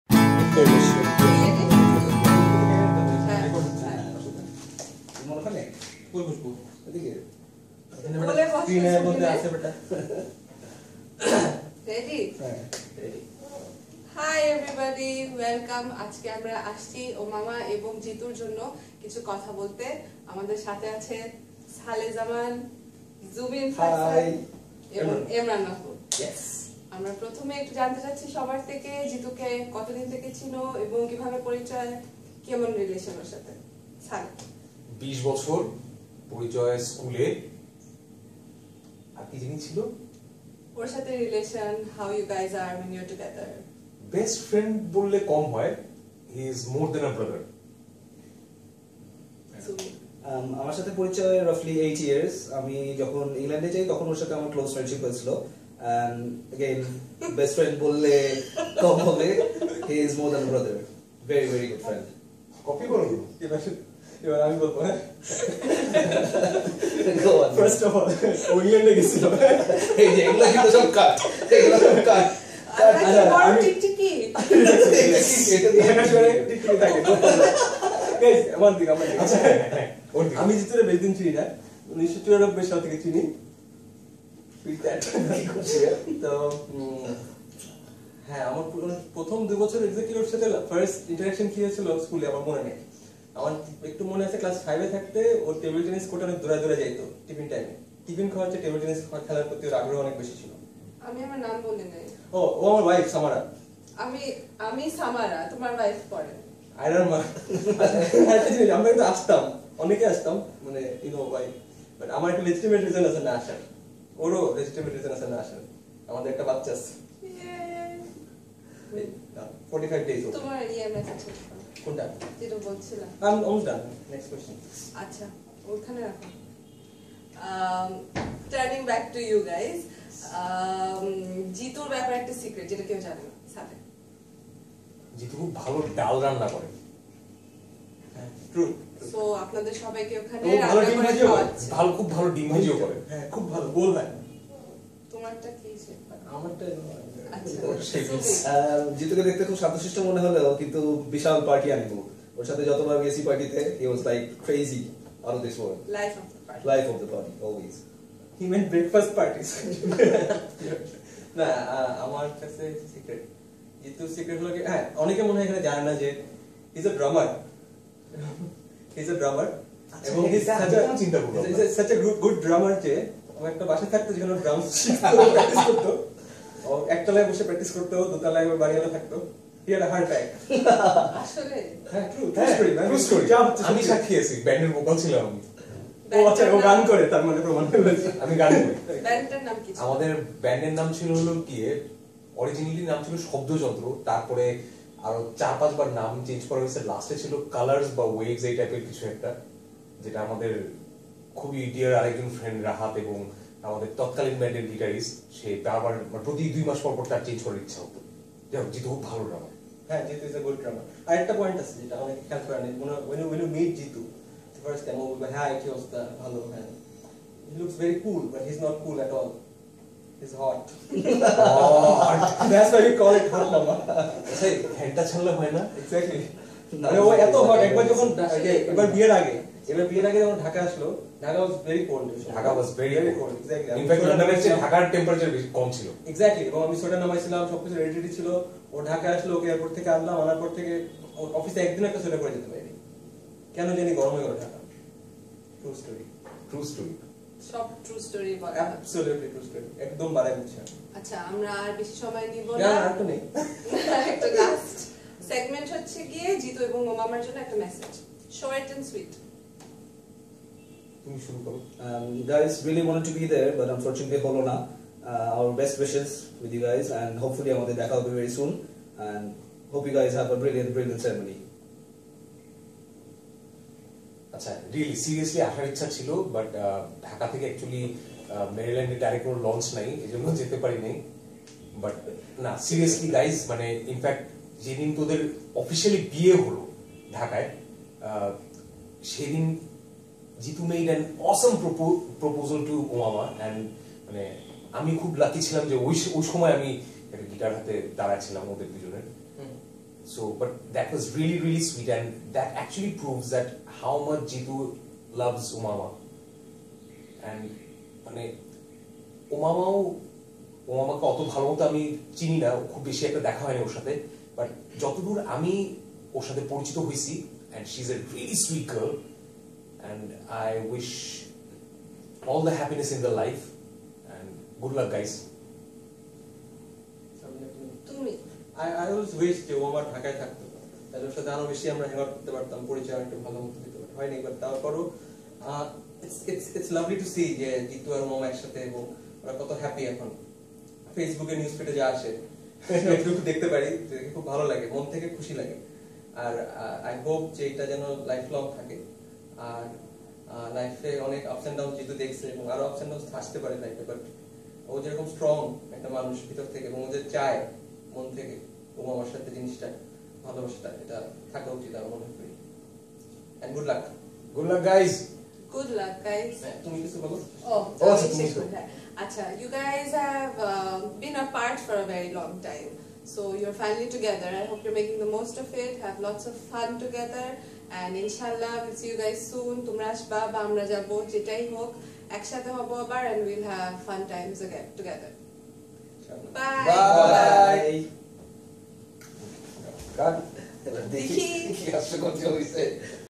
Friends' Video 🎥 Message at 👫 Jitu & Omama's Wedding Ceremony 💌 from Dhaka 🇧🇩
Saleh uz Zaman, Zubin Faisal, Emran Mahbub and Syeda Samara Mortada wishing Zubair Mahbub and Omama Younus on their wedding ceremony.